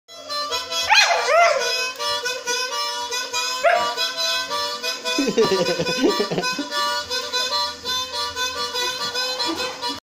Jess the dancing staffy
Blue staffy dancing with uncle cam